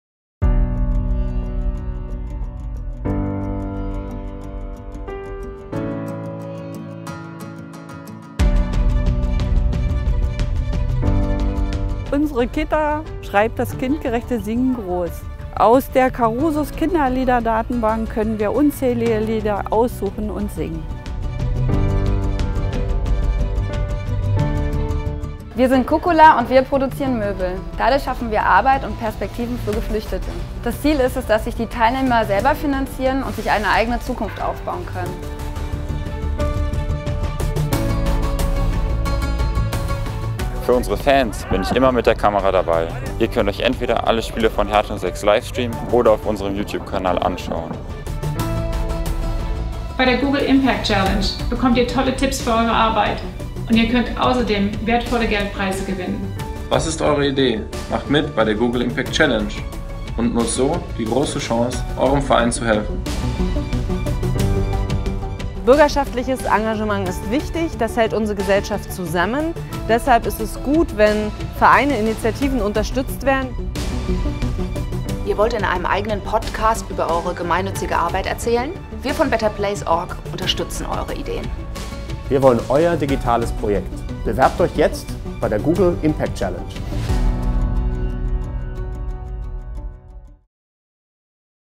Google Impact Challenge Deutschland
Google und betterplace.org rufen ab sofort alle „digitalen Helfer“ zur Teilnahme auf.

Ob die Erstellung einer Homepage für einen Fußballverein oder einer App, um Flüchtlingen zu helfen: Technologie kann gemeinnützige Organisationen in allen Bereichen unterstützen, die Welt etwas besser zu machen. Google hat deshalb heute mit seinen Partnern betterplace.org und Ashoka die „Google Impact Challenge“ gestartet ‒ zum ersten Mal in Deutschland.